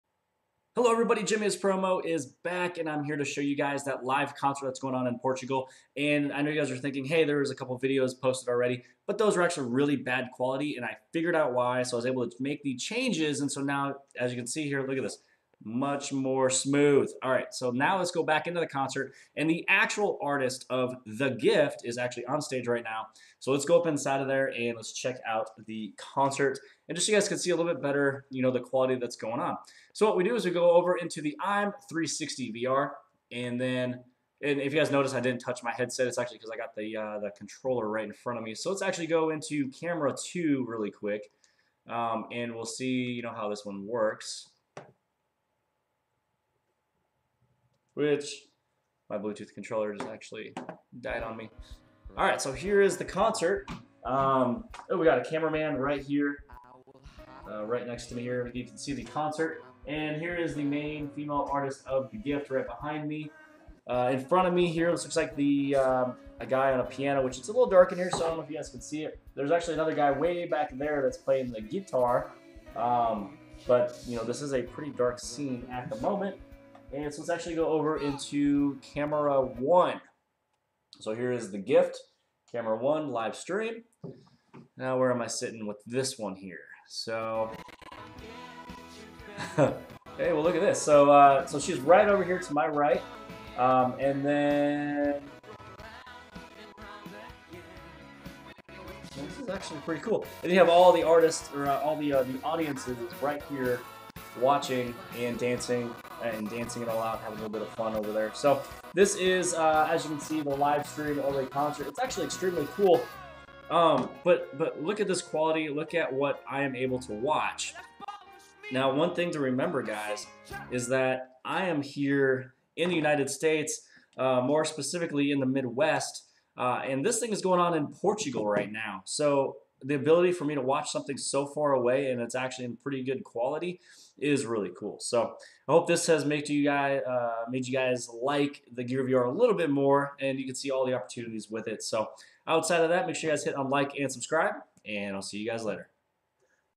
Samsung Gear VR - Live Stream of THE GIFT Concert
Here is a much better quality of the concert and the actual band. Happening in Portugal at the moment and hopefully will be saved in the Oculus store to view later. Enjoy!